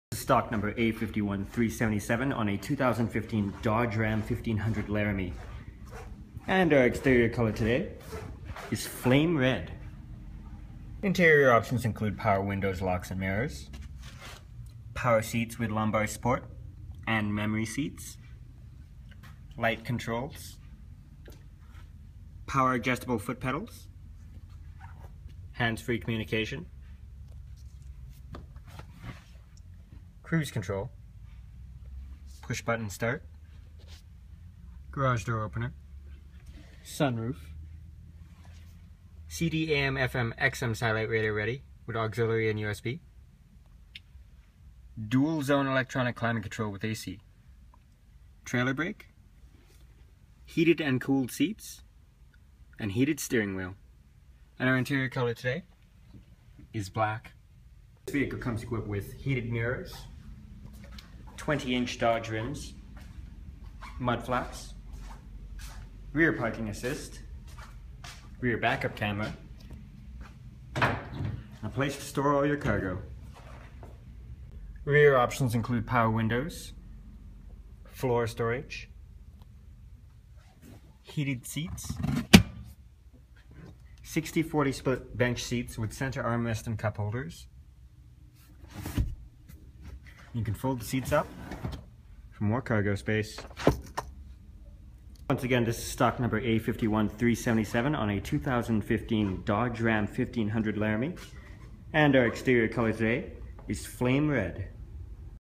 2015 Ram 1500 4WD Crew Cab 140.5" Laramie
This is a 2015 Ram 1500 4WD Crew Cab 140.5&quot; Laramie with Automatic transmission Red[PR4,Flame Red] color and Black interior color.  This video is recorded and uploaded by cDemo Mobile Inspector